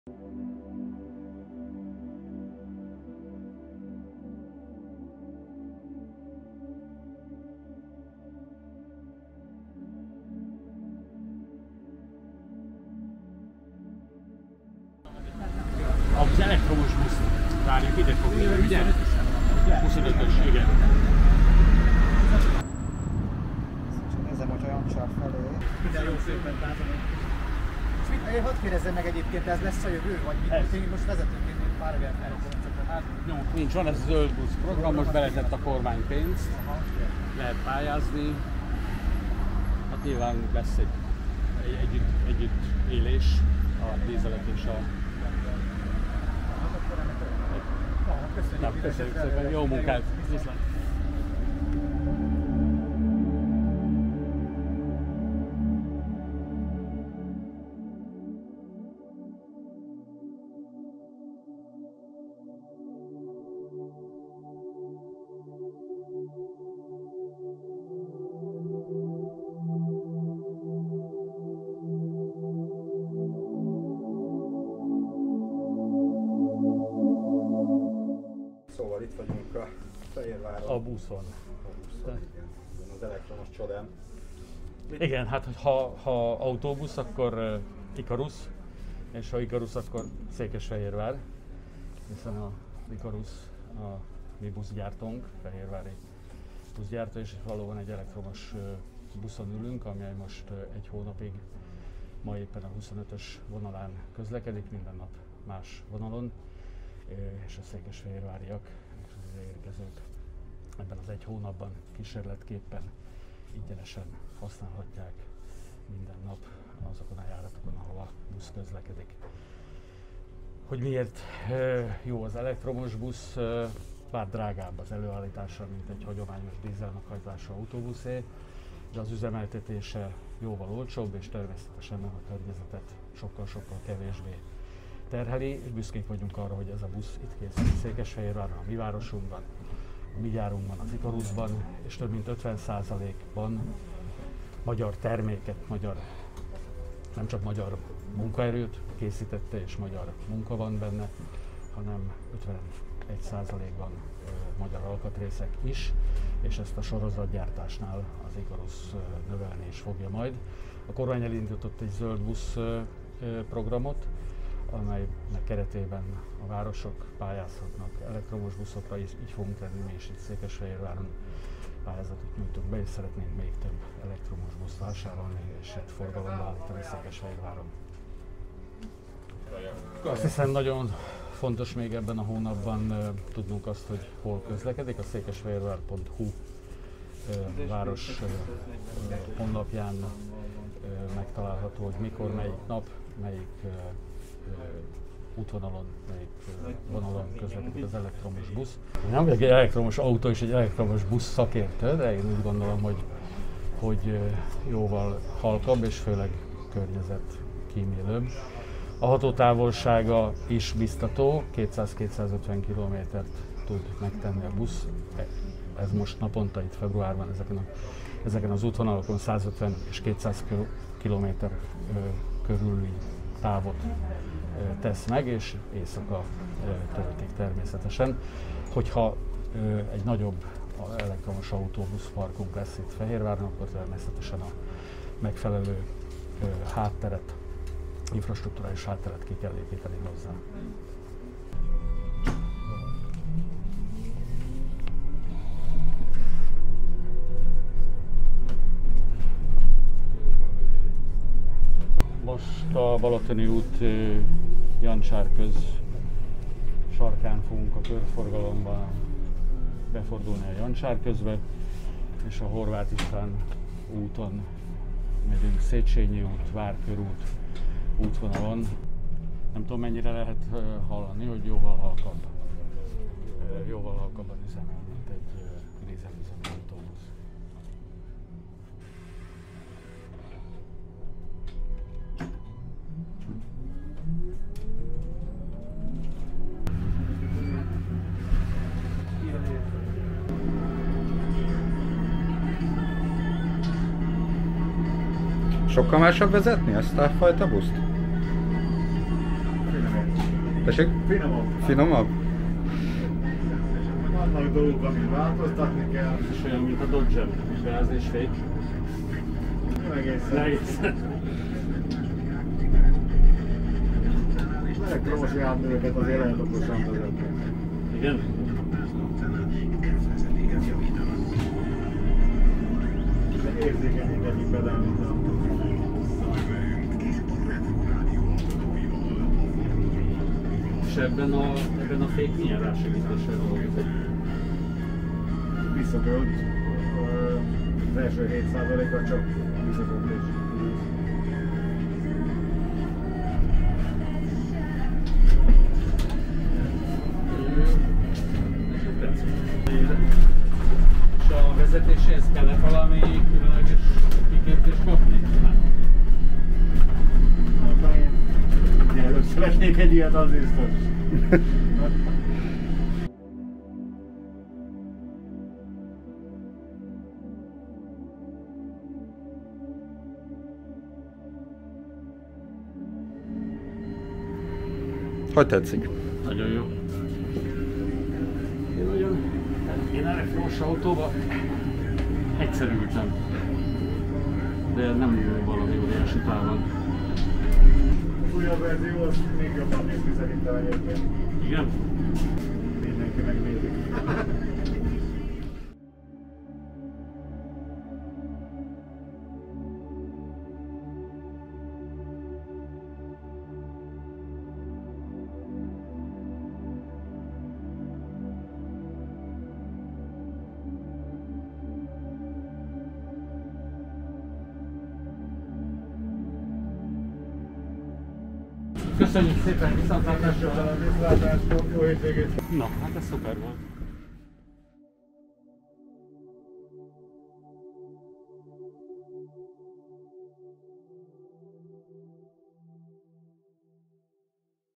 Kipróbáltuk a Székesfehérváron közlekedő elektromos buszt ⚡️🔋
A fehérvári Ikarusnál összeszerelt elektromos busz februárban közlekedik városunkban. A buszt bárki díjmentesen kipróbálhatja ebben az időszakban. Kipróbáltuk és erről készítettünk egy videót.

Önöknek hogy tetszik? Várjuk hozzászólásaikat a videó alatt!